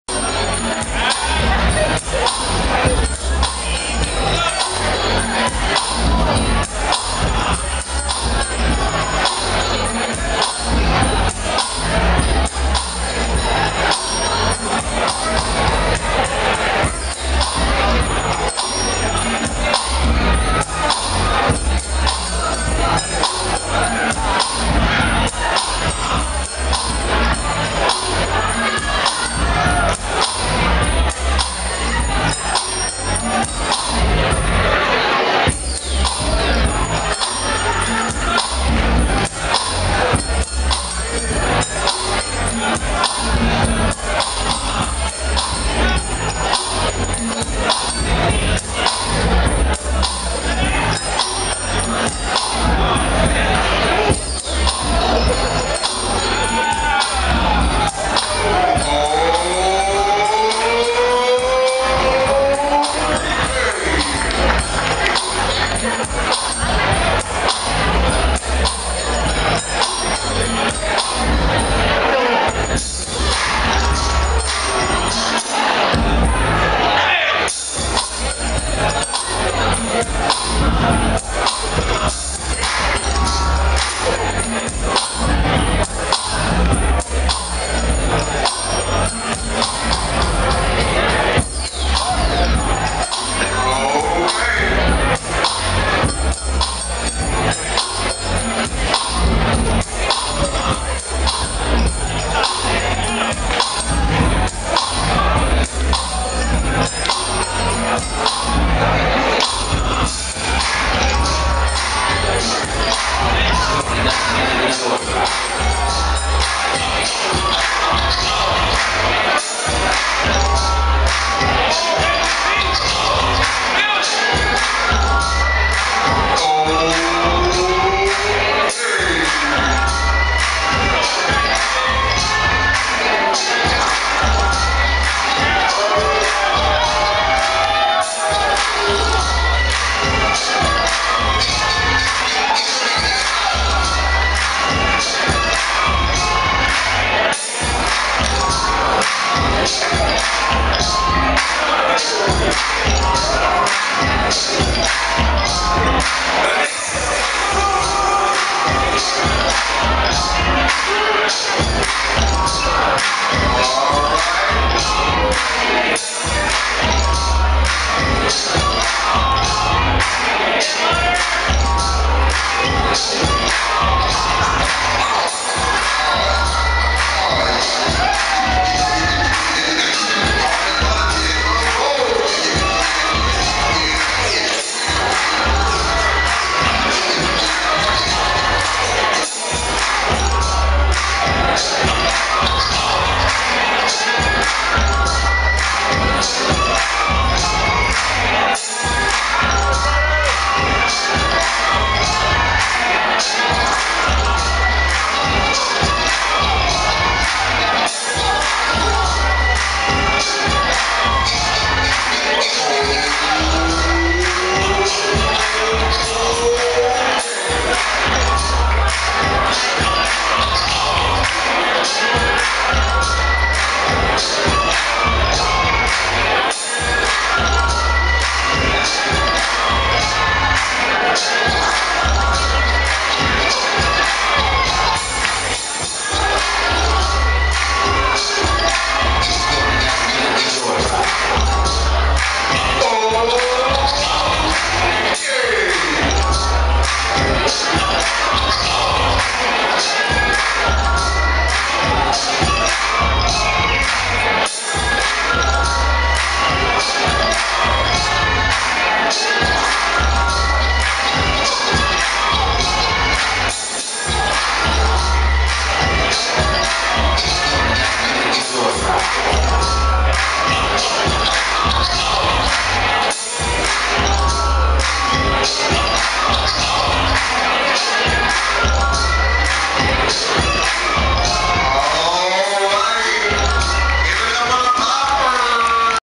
Popping Adam @ Juste Debout NYC 2011 - Popping Prelims (USA Qualifiers)
**IM AT 3:40 !

Popping Adam @ Juste Debout NYC 2011 - Popping Prelims 8 (USA Qualifiers)